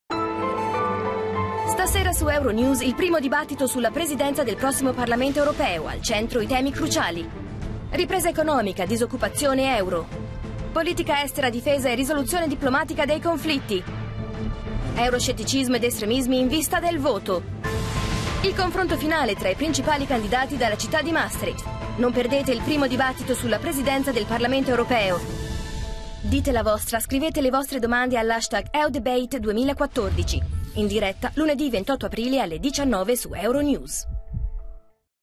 EUdebate2014 - Segui il dibattito presidenziale europeo, il 28 aprile alle 19 ora centrale europea
Il 28 aprile, alle 19:00, ora centrale europea, quattro candidati alla prossima presidenza della Commissione europea, a Maastricht, si affronteranno nel primo dibattito televisivo. Il confronto sarà trasmesso in diretta su euronews.

In collaborazione con lo European Youth Forum e l'Università di Maastricht, nei Paesi Bassi, Jean-Claude Juncker (Partito Popolare Europeo), Martin Schulz (Partito Socialista Europeo), Guy Verhofstadt (Alleanza dei Democratici e dei Liberali per l'Europa) e Ska Keller (Partito dei Verdi europei) si daranno battaglia su questioni fondamentali per il futuro dell'Europa.

Moderato da Isabelle Kumar e Chris Burns, giornalisti di euronews,
il dibattito, davanti a una platea di 700 giovani, durerà 90 minuti e sarà trasmesso da euronews in tutto il mondo, tradotto in tredici lingue.

Il confronto, che prevede domande del pubblico presente in studio e della comunità virtuale, verterà sui temi più importanti per i giovani europei. Mark Davis, responsabile dei contenuti digitali, sarà la voce delle domande ricevute all'hashtag EUdebate2014.

Per la prima volta, sotto lo slogan 'è diverso' i gruppi politici Europei
presentano i loro candidati alla presidenza, dando agli elettori voce in capitolo su chi potrebbe essere il leader dell'esecutivo dell'Unione europea.

Avete qualcosa da chiedere al potenziale nuovo presidente della Commissione europea? Mandateci le vostre domande usando l'hashtag EUdebate2014 su Twitter, Facebook o Google+.